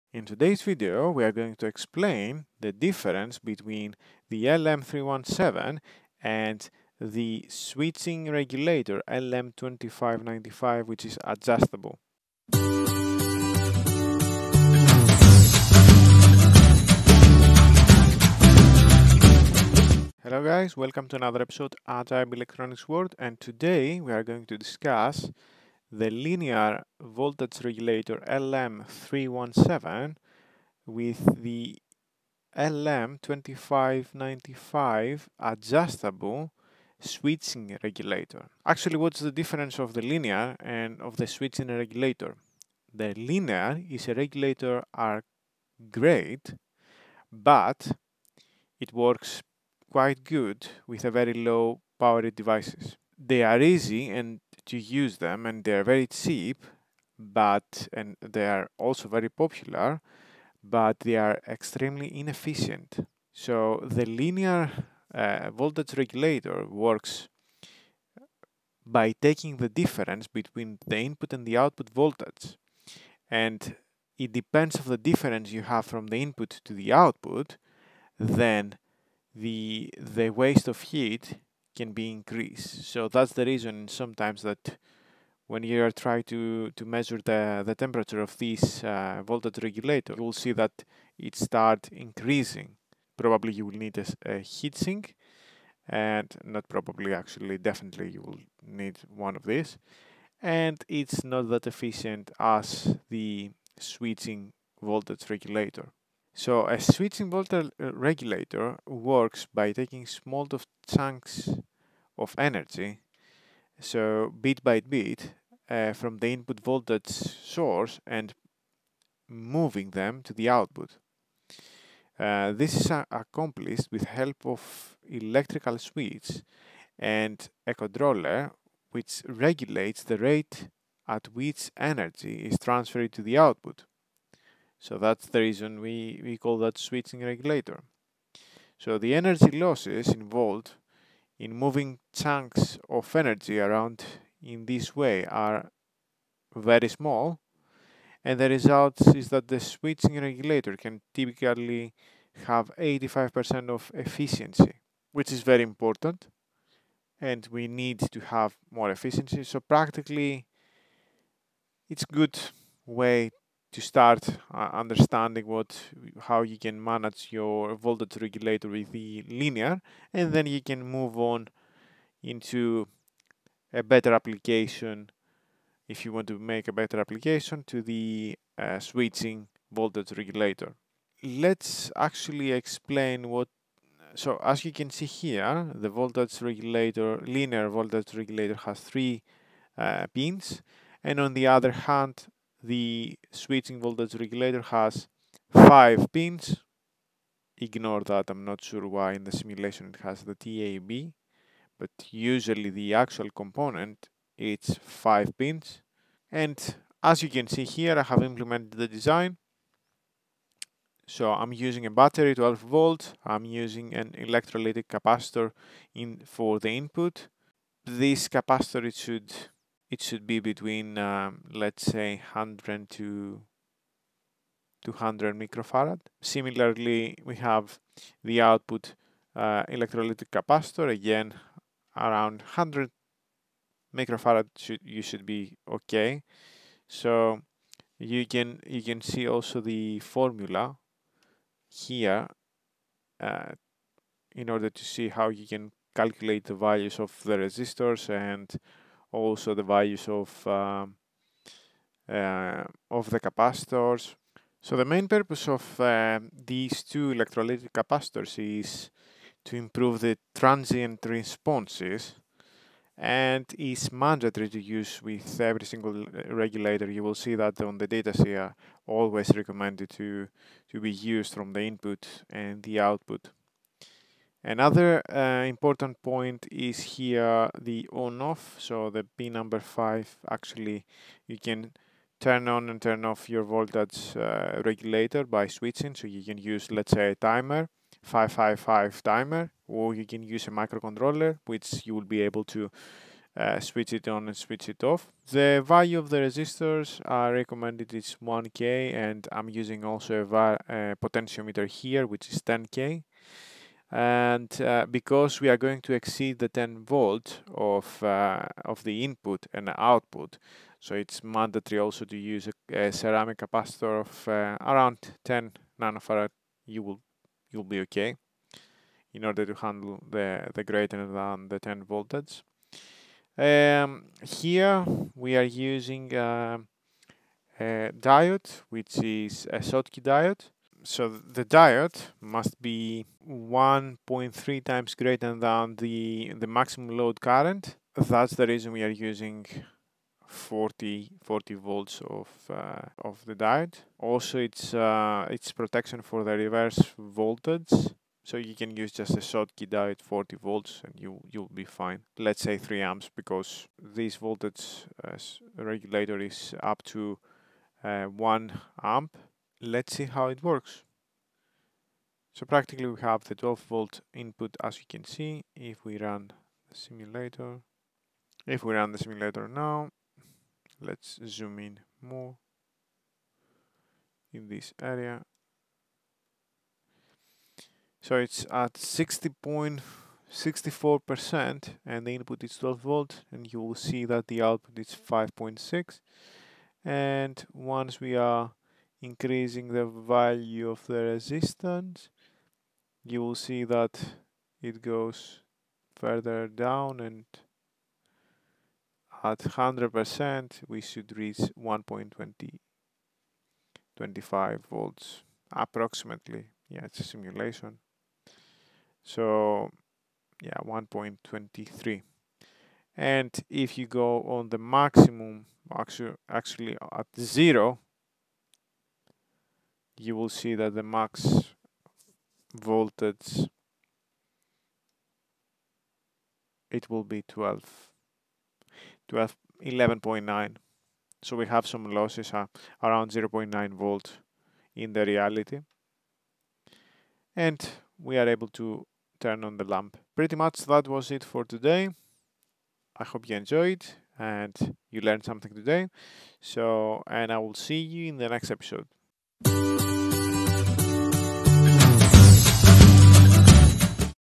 How to use switching regulator LM2595
This is a tutorial that explains how you can use the switching voltage regulator. In addition, you will be able to understand the difference between the linear voltage regulator and the switching voltage regulator.